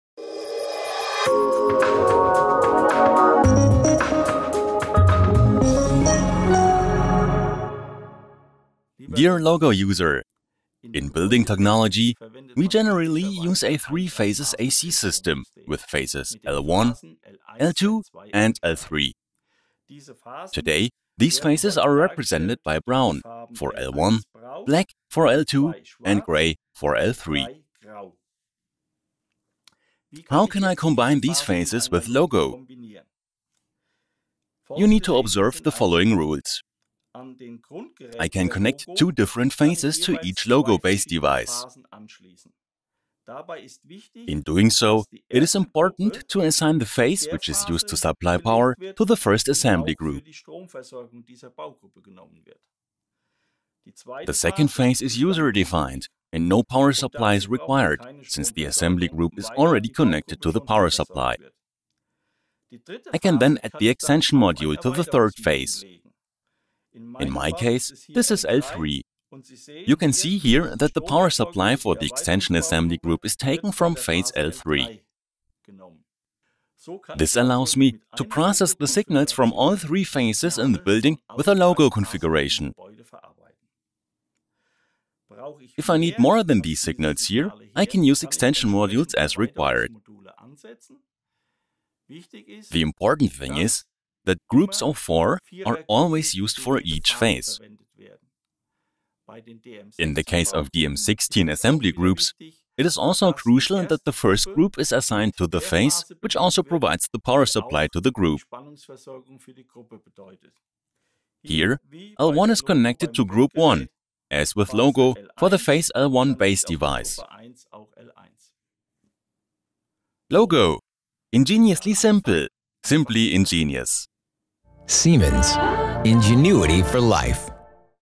LOGO! Tips and Tricks - Wiring (EN)
LOGO! Simply ingenious for small automation tasks.
This video shows you some tips and tricks to LOGO! and LOGO! Soft Comfort for an even more efficient and more comfortable use.